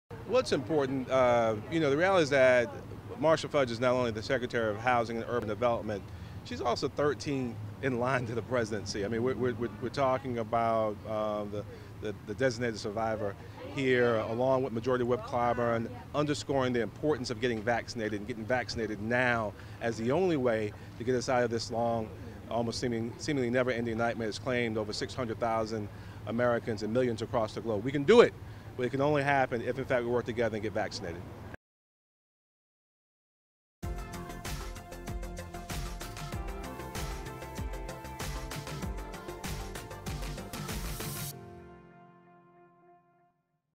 Mayor Benjamin | Why Secretary Fudge's Visit to Columbia is so Important
The U.S. Department of Housing and Urban Development Secretary, Marcia L. Fudge, traveled to Columbia to highlight the importance of vaccinations and to discuss housing-related issues.

Secretary Fudge was joined by U.S. Congressman James E. Clyburn, Mayor Benjamin and other elected officials during the visit.